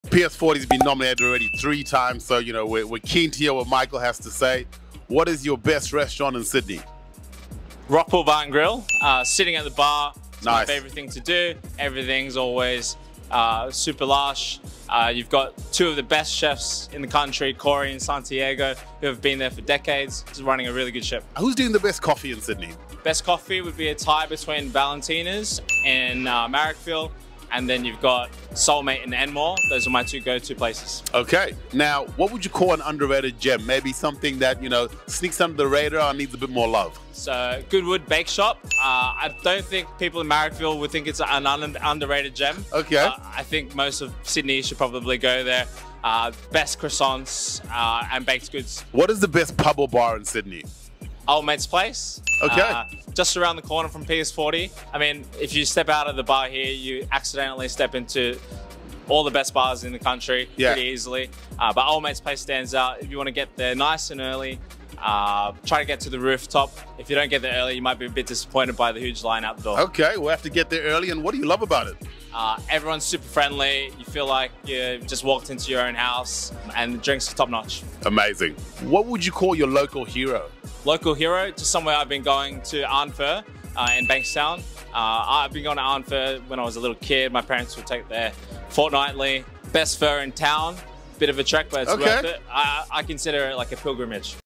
Michael Chiem, Owner of PS40 shares his best of Sydney
Best Restaurant - Rockpool Bar & Grill
Best Coffee - Soulmate Coffee
Best Coffee - Valentinas Sydney
Underrated Gem - Goodwood Bakeshop Marrickville
Go-to Pub/Bar - Old Mates Place